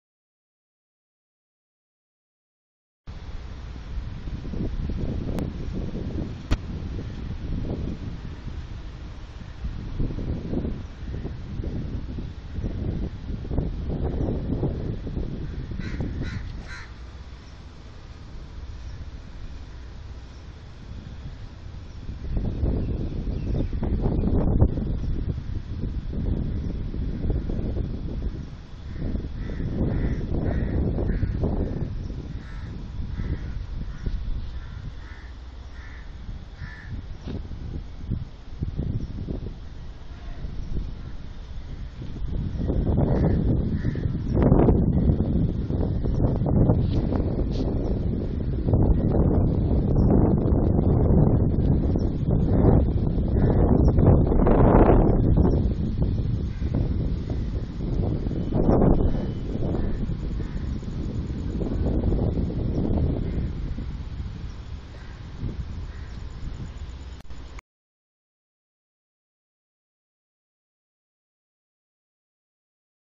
JCB and Concrete Boom Placer
JCB and Boom Placer one of the famous Machine in Construction site